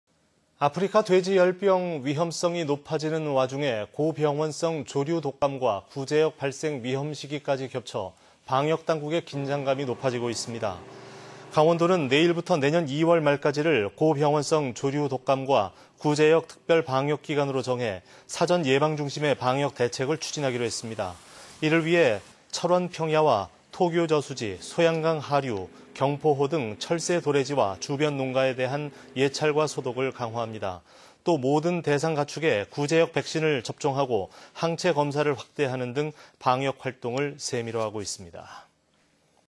[단신] 강원도, 고병원성 AI.구제역 특별 방역 추진 190930
아프리카 돼지열병 위험성이 높아지는 와중에 고병원성 조류독감과 구제역 발생 위험시기까지 겹쳐 방역 당국의 긴장감이 높아지고 있습니다.
           ◀ＥＮＤ▶
 강원도는 내일(1일)부터 내년 2월 말까지를
고병원성 조류독감과 구제역 특별방역기간으로 정해, 사전 예방 중심의 방역 대책을 추진하기로 했습니다.

 이를 위해 철원 평야와 토교 저수지, 소양강
하류, 경포호 등 철새 도래지와 주변 농가에
대한 예찰과 소독을 강화합니다.

 또 모든 대상 가축에 구제역 백신을 접종하고, 항체 검사를 확대하는 등
방역 활동을 세밀화하고 있습니다.//